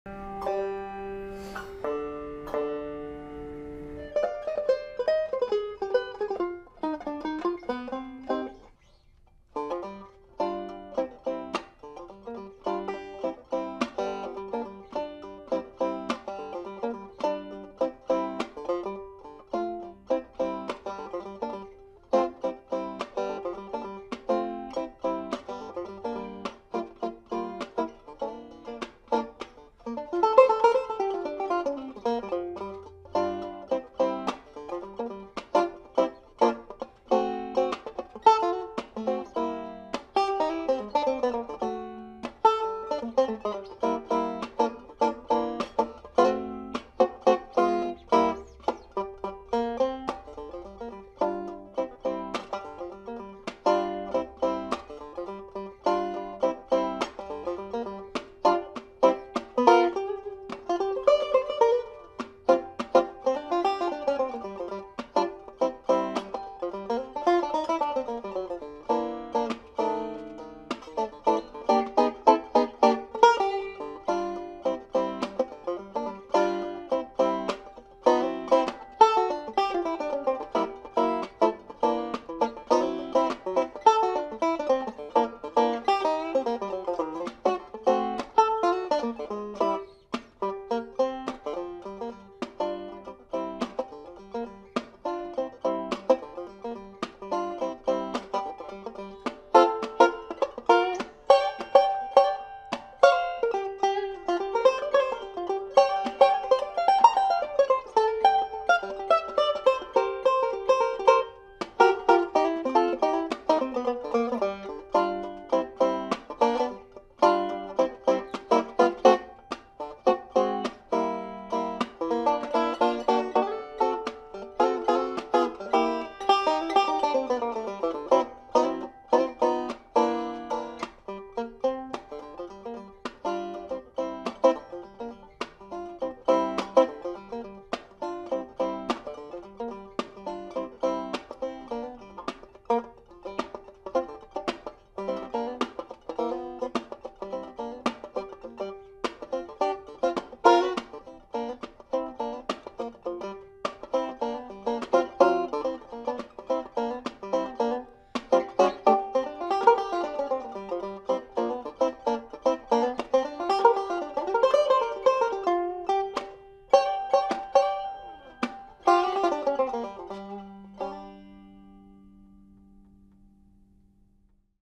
Deering Eagle II Banjo with Ryan Cavanaugh | Improvised Funk Jam
Ryan Cavanaugh takes the Deering Eagle II banjo for a test drive through his wonderful world of improvisation, this time exploring his funk tendencies.

I don't know about you, but the Eagle II sounds like a great funk instrument!